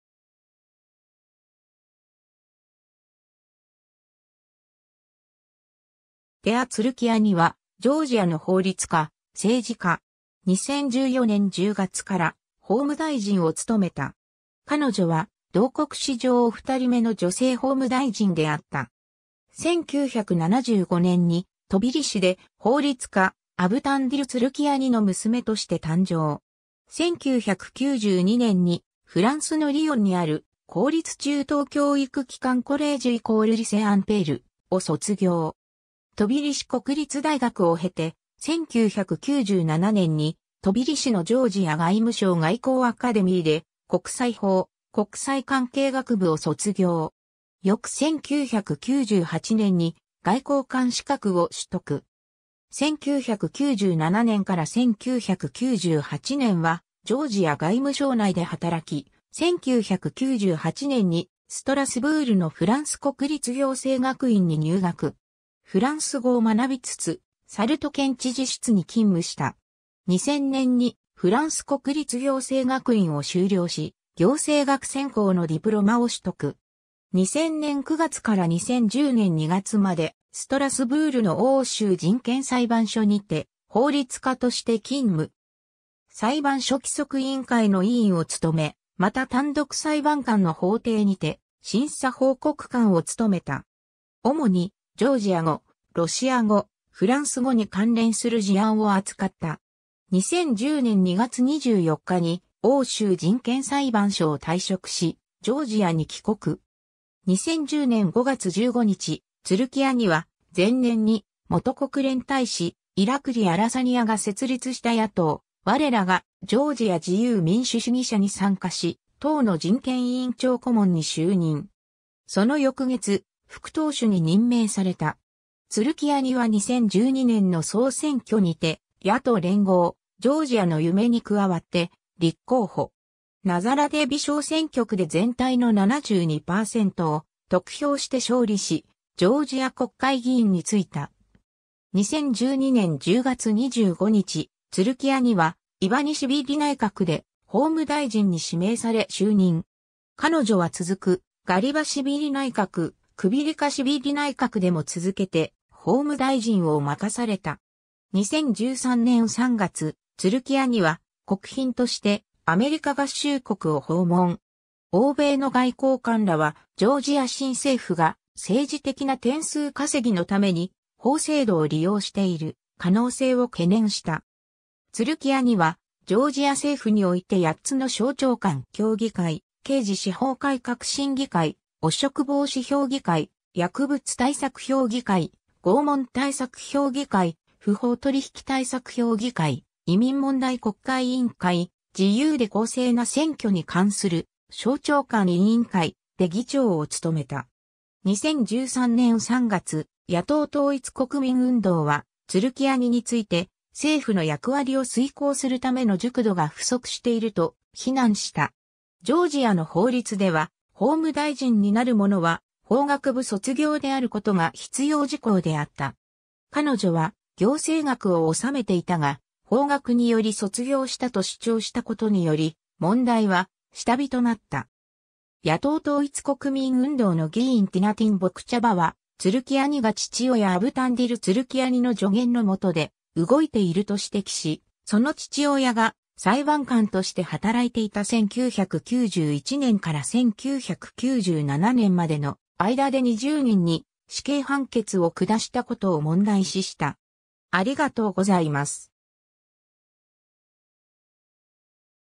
テア・ツルキアニ
テア・ツルキアニ（グルジア語: თეა წულუკიანი、グルジア語ラテン翻字: Tea Tsulukiani、1975年1月21日 – ）は、ジョージアの法律家、政治家。
2014年10月から法務大臣を務めた。
彼女は同国史上2人目の女性法務大臣であった。
 1975年にトビリシで法律家アヴタンディル・ツルキアニの娘として誕生。
1992年にフランスのリヨンにある公立中等教育機関コレージュ＝リセ・アンペールを卒業。
トビリシ国立大学を経て、1997年にトビリシのジョージア外務省外交アカデミーで国際法・国際関係学部を卒業。
翌1998年に外交官資格を取得。
1997年から1998年はジョージア外務省内で働き、1998年にストラスブールのフランス国立行政学院に入学。
フランス語を学びつつ、サルト県知事室に勤務した。
2000年にフランス国立行政学院を修了し、行政学専攻のディプロマを取得。
 2000年9月から2010年2月までストラスブールの欧州人権裁判所にて法律家として勤務。
裁判所規則委員会の委員を務め、また単独裁判官の法廷にて審査報告官を務めた。
主にジョージア語、ロシア語、フランス語に関連する事案を扱った。
2010年2月24日に欧州人権裁判所を退職し、ジョージアに帰国。
 2010年5月15日、ツルキアニは前年に元国連大使イラクリ・アラサニアが設立した野党「我らがジョージア 自由民主主義者」に参加し、党の人権委員長顧問に就任。
その翌月、副党首に任命された。
ツルキアニは2012年の総選挙にて、野党連合「ジョージアの夢」に加わって立候補。
ナザラデヴィ小選挙区で全体の72パーセントを得票して勝利し、ジョージア国会議員に就いた。
 2012年10月25日、ツルキアニはイヴァニシヴィリ内閣で法務大臣に指名され就任。
彼女は続くガリバシヴィリ内閣、クヴィリカシヴィリ内閣でも続けて法務大臣を任された。
2013年3月、ツルキアニは国賓としてアメリカ合衆国を訪問。
欧米の外交官らは、ジョージア新政府が政治的な点数稼ぎのために法制度を利用している可能性を懸念した。
 ツルキアニはジョージア政府において8つの省庁間協議会――刑事司法改革審議会、汚職防止評議会、薬物対策評議会、拷問対策評議会、不法取引対策評議会、移民問題国家委員会、自由で公正な選挙に関する省庁間委員会――で議長を務めた。
 2013年3月、野党統一国民運動はツルキアニについて、政府の役割を遂行するための熟度が不足していると非難した。
ジョージアの法律では、法務大臣になる者は法学部卒業であることが必要事項であった。
彼女は行政学を修めていたが、法学により卒業したと主張したことにより、問題は下火となった。
 野党統一国民運動の議員ティナティン・ボクチャヴァは、ツルキアニが父親アヴタンディル・ツルキアニの助言の下で動いていると指摘し、その父親が裁判官として働いていた1991年から1997年までの間で20人に死刑判決を下したことを問題視した。